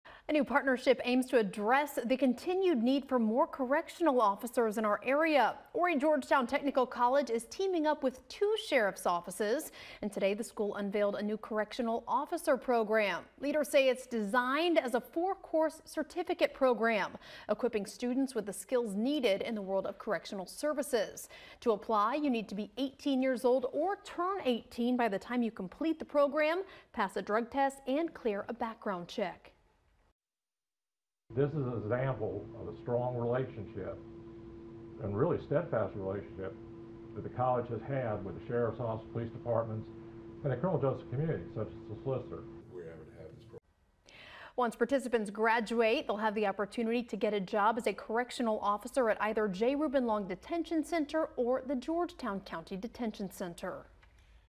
Horry-Georgetown Technical College announces correctional officer program
HGTC is teaming up with local law enforcement to train the next generation of corrections officers.